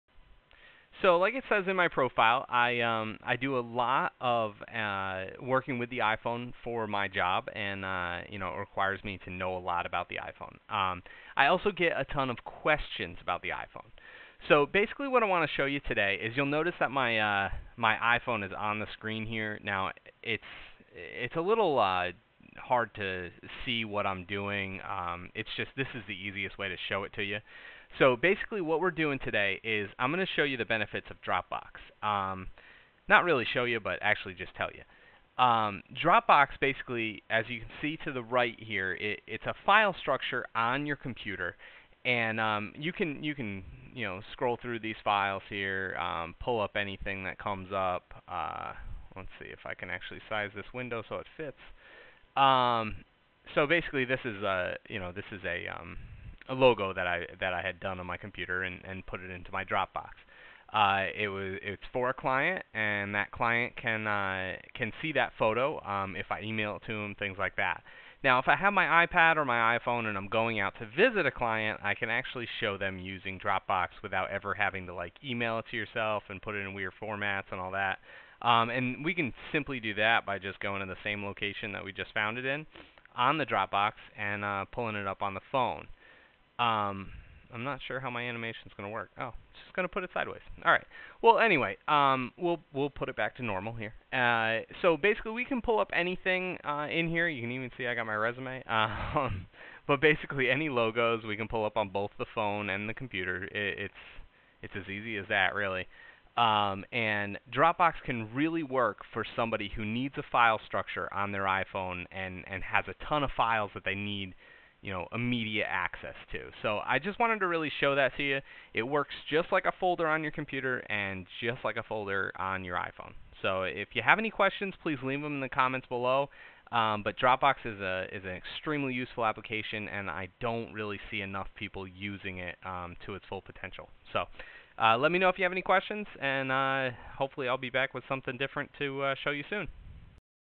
Using Dropbox on iPhone iPad or iPod touch
Quick video on how to use drop box with an iPhone, iPad or iPod touch